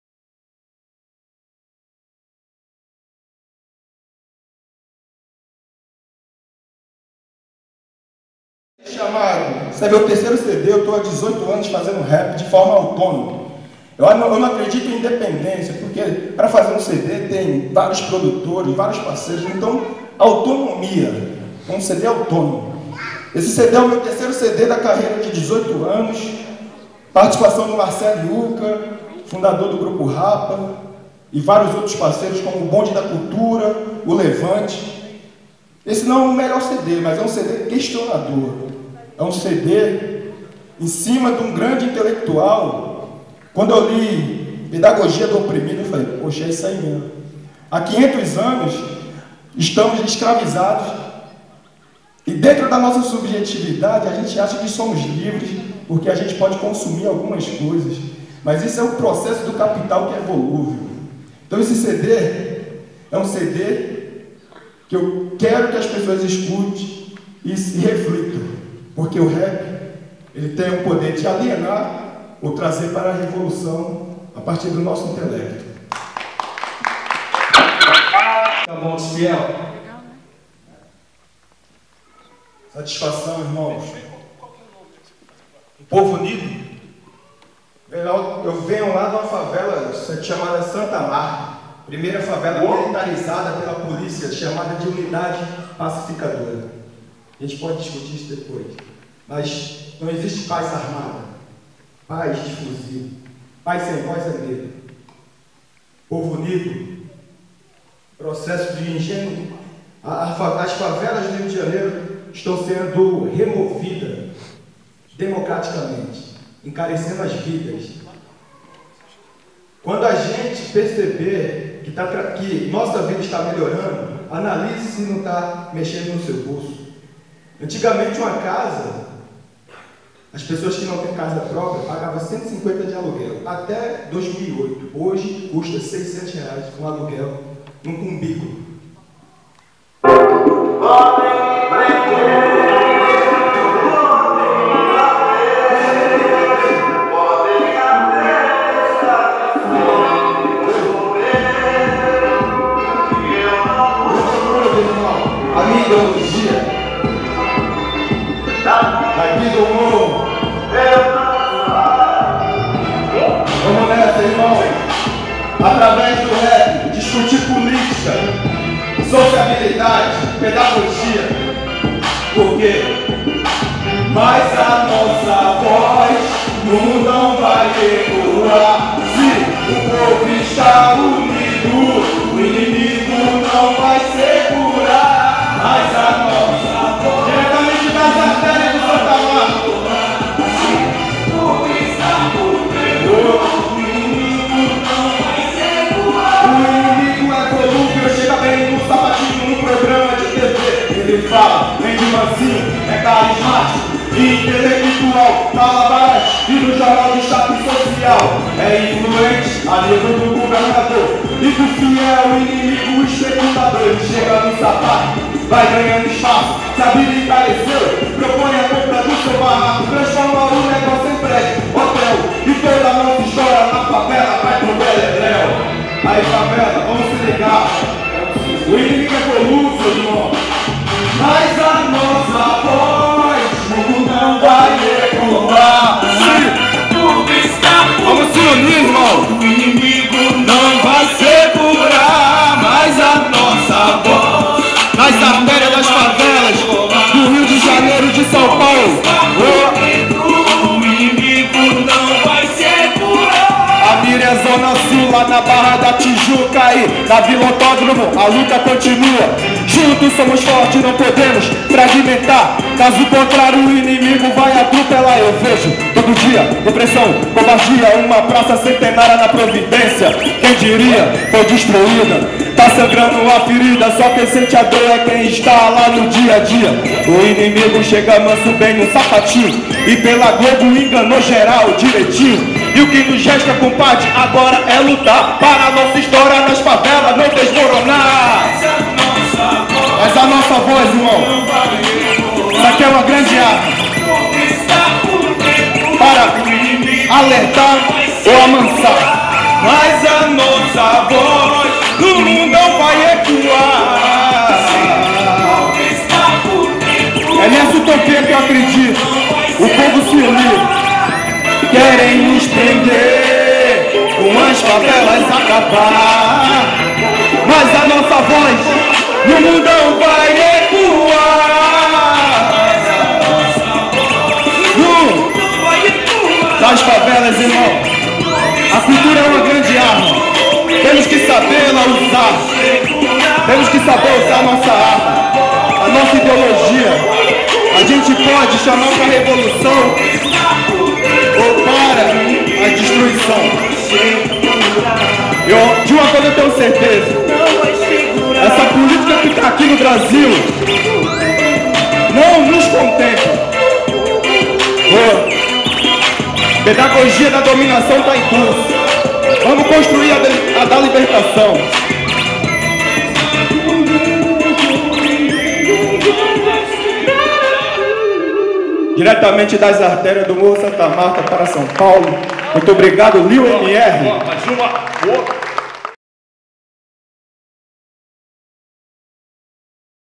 Repper Fiell no Céu Heliópolis - São Paulo. 25/08/2013.
Liu MR convida o repper Fiell para participar do seu show no Céu de Heliópolis.